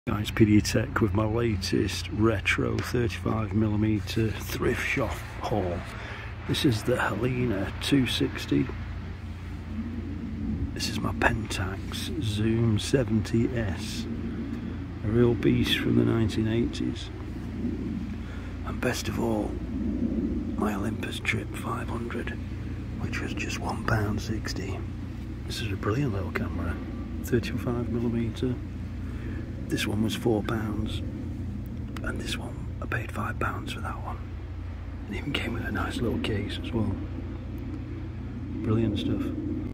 My Latest Retro 35mm Camera Thrift Shop £10 Haul
My latest thrift shop £10 haul included an Olympus Trip 500 for £1.60 and a fantastic 1980s Pentax zoom 70s for £4 what a bargain, these take stunning photos.